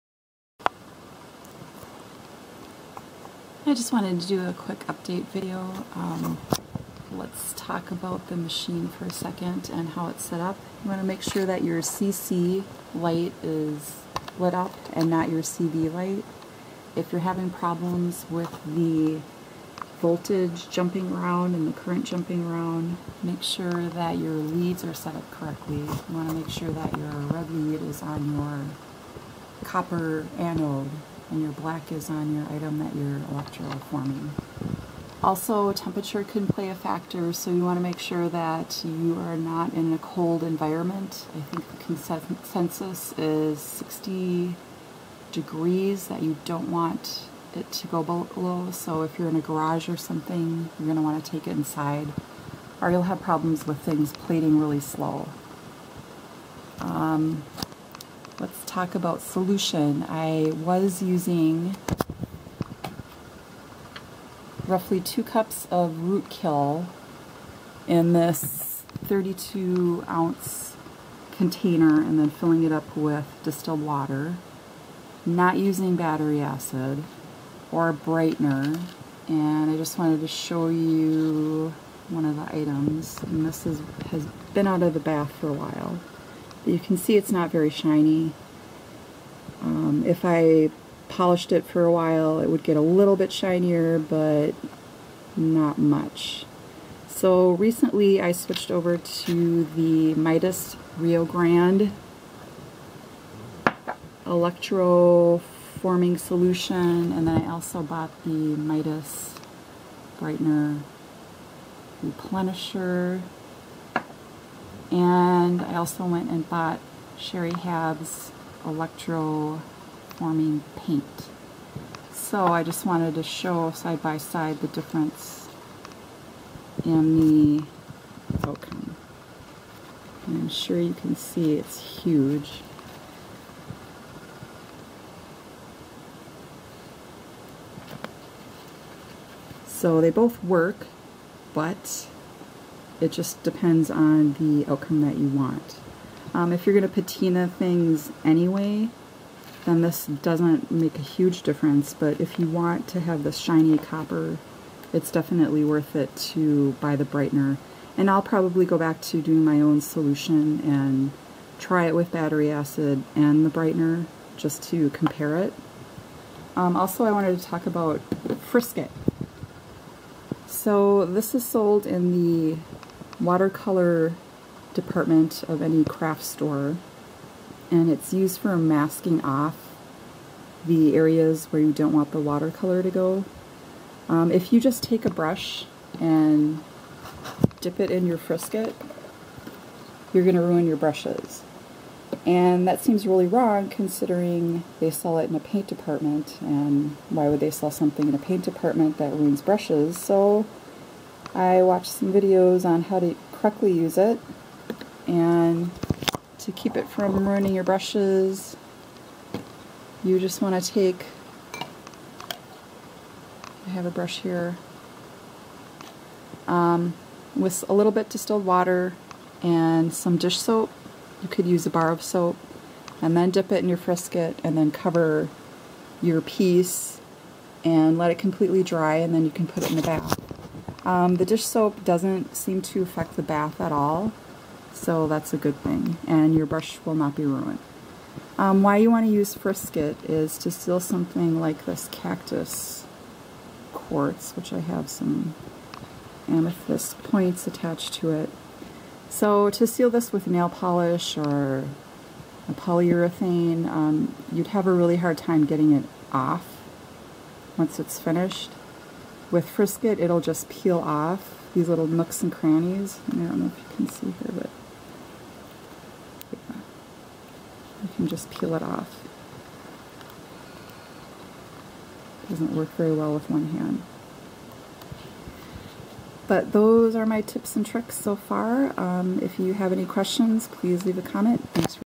More Electroforming Tips
Rectifier trouble shooting, frisket tips  and home made solution vs. Midas brand with brightener.